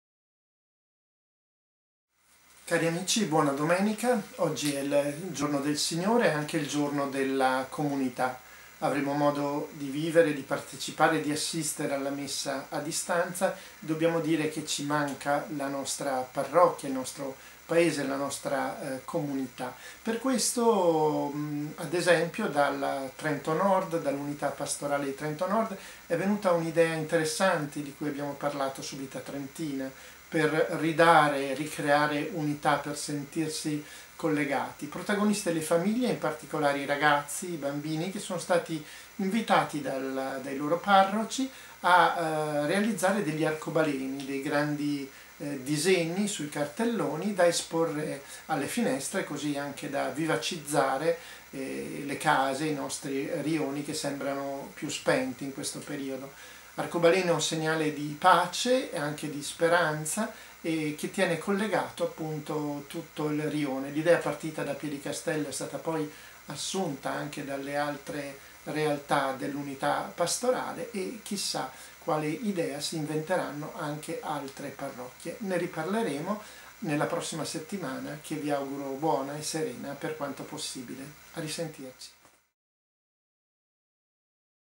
Arcobaleni colorati alle finestre per sentirsi vicini
Tanti arcobaleni ai balconi e alle finestre: è partito da Piedicastello l’invito diffuso dalle parrocchie di Trento Nord per sentirsi più vicini, nella distanza di questo momento. Ce lo racconta il direttore Diego Andreatta.